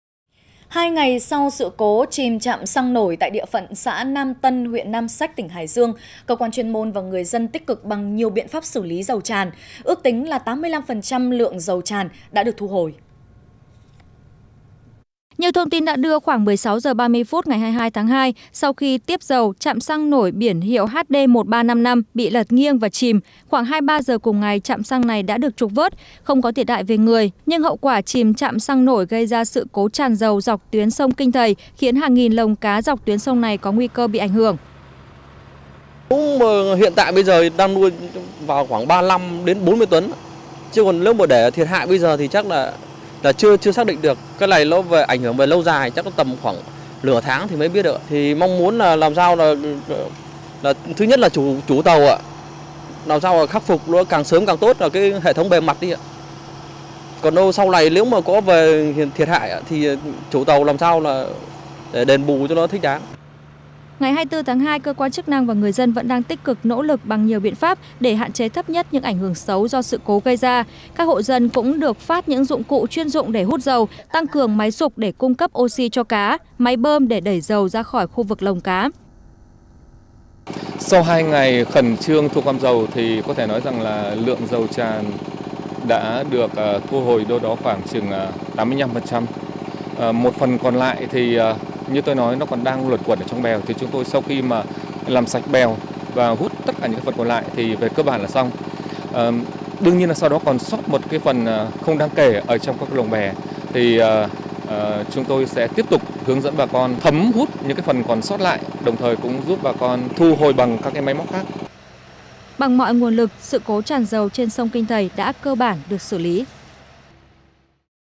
TH Nhân Dân - Thời sự: Sự cố tràn dầu tại huyện Nam Sách, tỉnh Hải Dương (24/02/2019)
Hoạt động ứng phó sự cố tràn dầu của Trung tâm Ứng phó sự cố môi trường Việt Nam (Trung tâm SOS) tại Nam Sách, Hải Dương